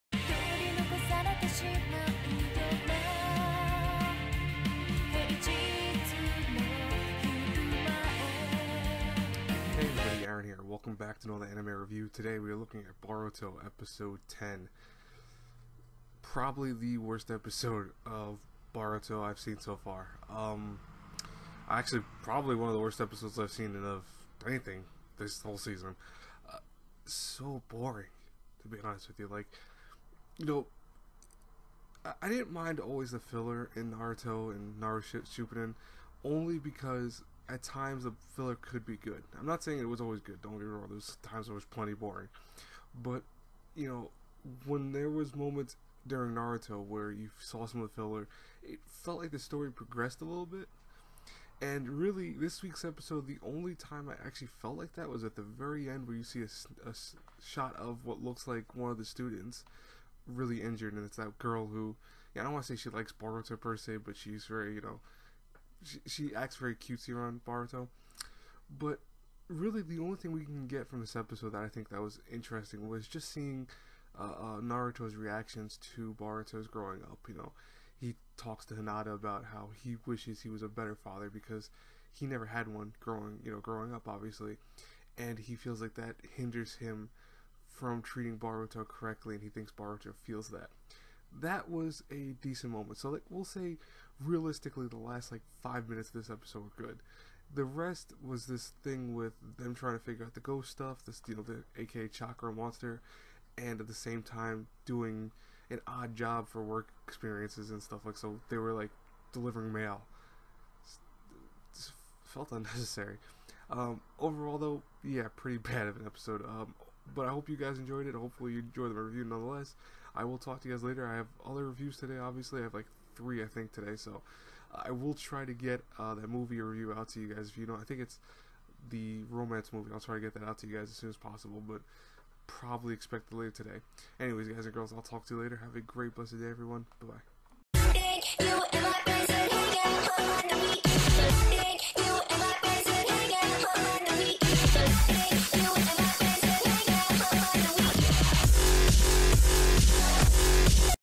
Boruto: Naruto next generations Episode 10 Review/Impressions | Boring as hell
Boruto: Naruto next generations Episode 10 was one simple word, Boring. Seriously this week almost put me back to sleep, while the ending 5 minutes were solid, the rest was horrid. Also I apologize for the low volume.